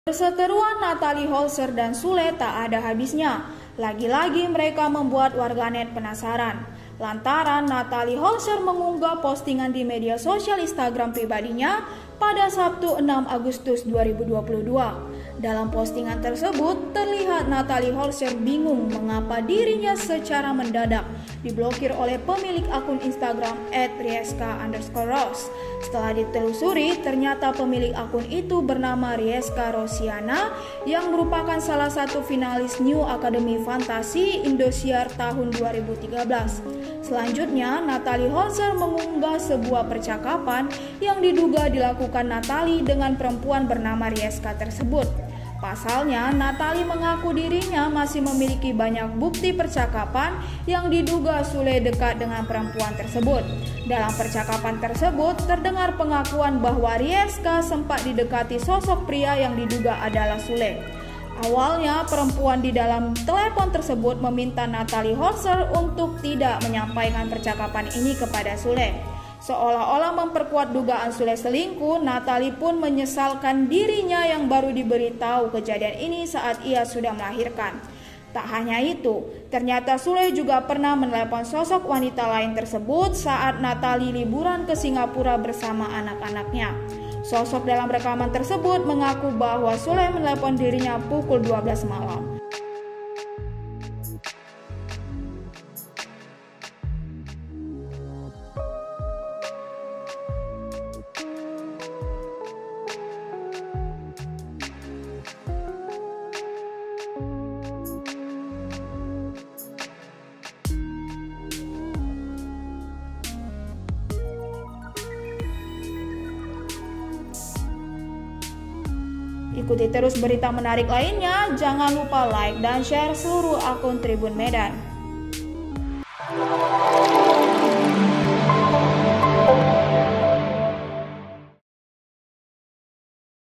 TERBONGKAR Nathalie Holscher Posting Bukti Percakapan Sule, Tak Sangka Ini Penyebabnya
TRIBUN-MEDAN.com – Perseteruan Nathalie Holscher dan Sule tak ada habisnya, lagi-lagi mereka membuat warganet penasaran.

Dalam postingan tersebut, terlihat Nathalie Holscher bingung mengapa dirinya secara mendadak diblokir oleh pemilik akun instagram @riessca_rose.